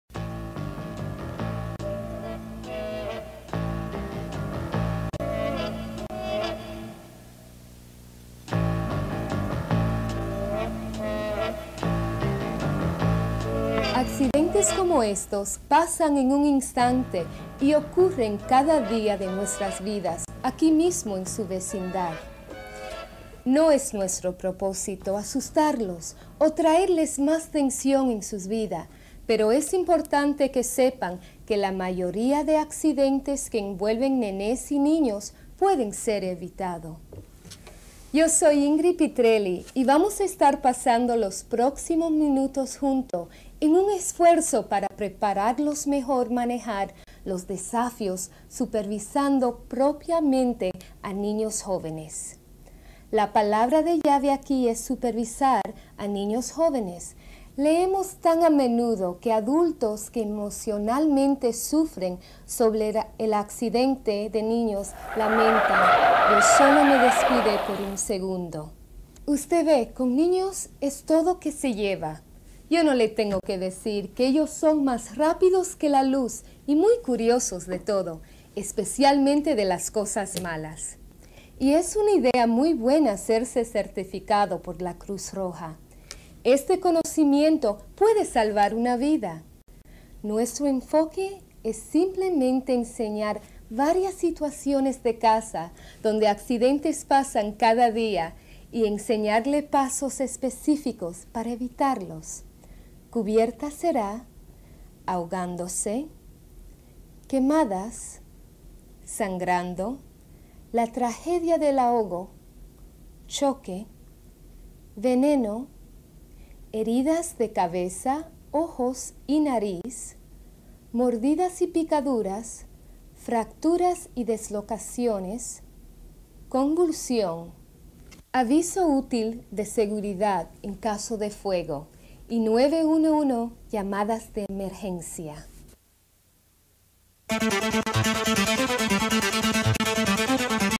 Child Safety at Home in Spanish
There are some basic steps that a parent or in-home caregiver can take to prevent injuries to small children in the home. Child Safety at Home video provides safety tips and first aid education in basic language and easy-to-follow video demonstrations. This program is ideal for young new parents, nannies and babysitters. Seasoned working parents can also benefit from this classic video.

Contents are based on the American Academy of Pediatrics (AAP) standards on child safety.

Also Available in Spanish
See also, Child Safety Outdoors, video.

Topics Covered Include:

• Choking
• Drowning
• Burns
• Shocks
• Convulsions
• And more